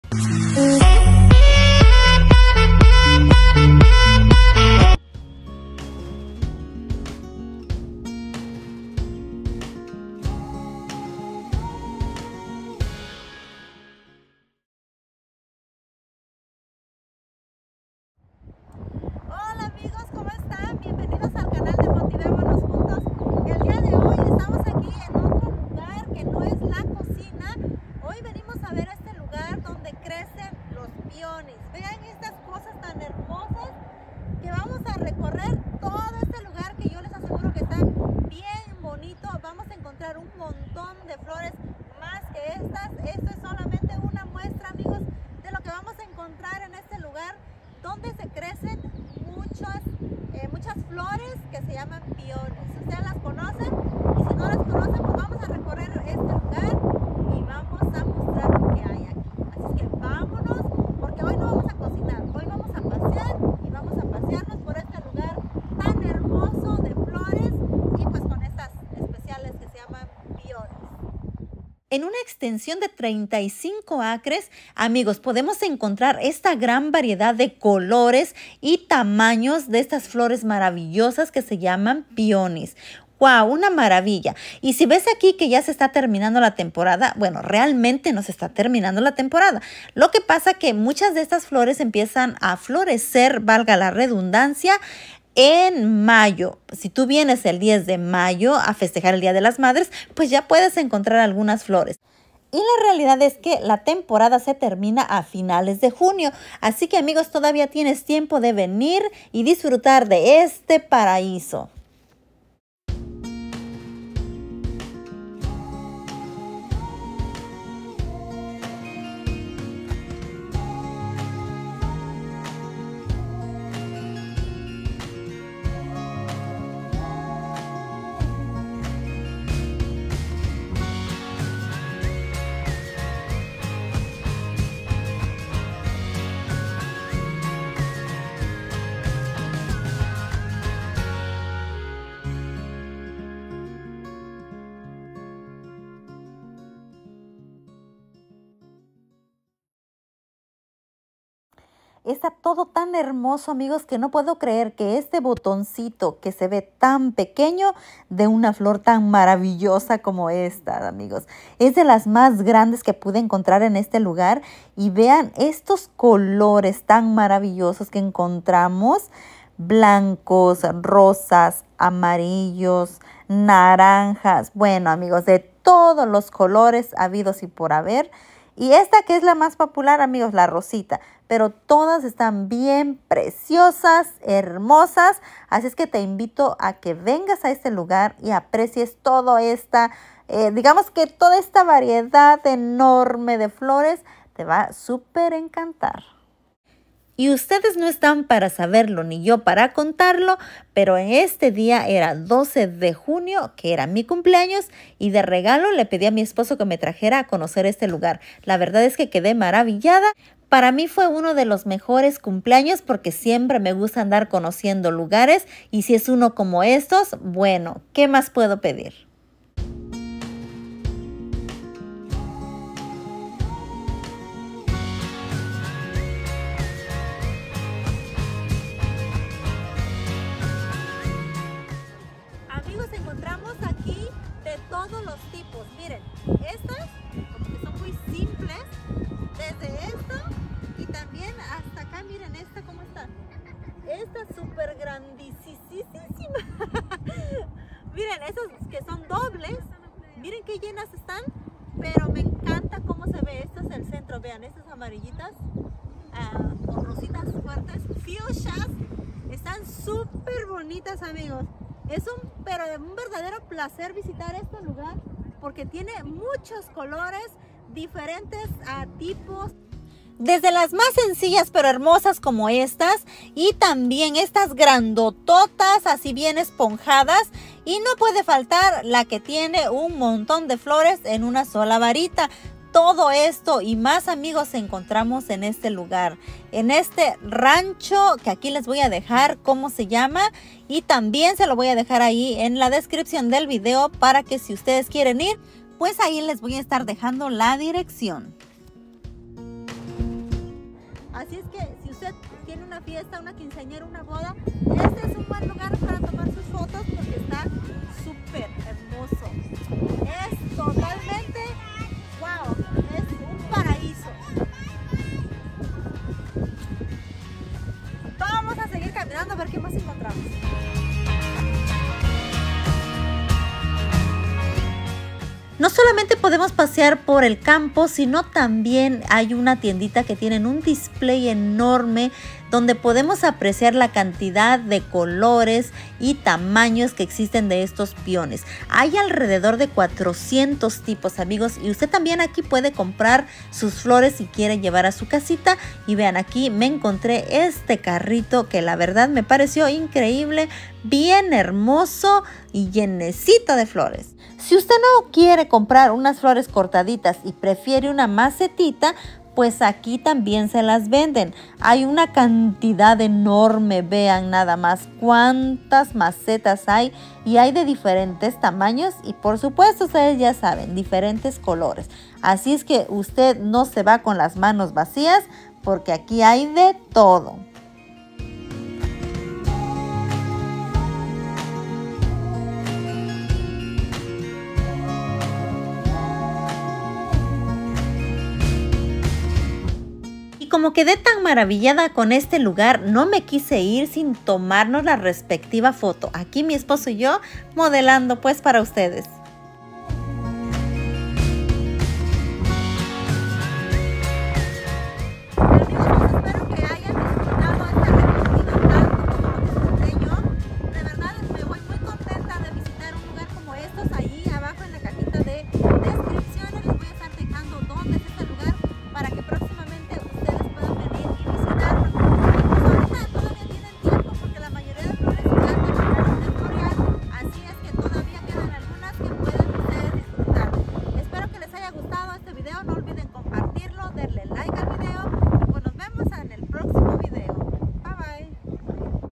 Ven y conoce este paraíso cosecha de peonies
Amigos los
Invito a recorrer este lugar maravilloso encontrarás hermosos peonies de todos colores y tamaños aquí te dejo la dirección
Lugar : Adelman peonies
5690 brooklake rod NE Salem Oregón
Ojalá te guste el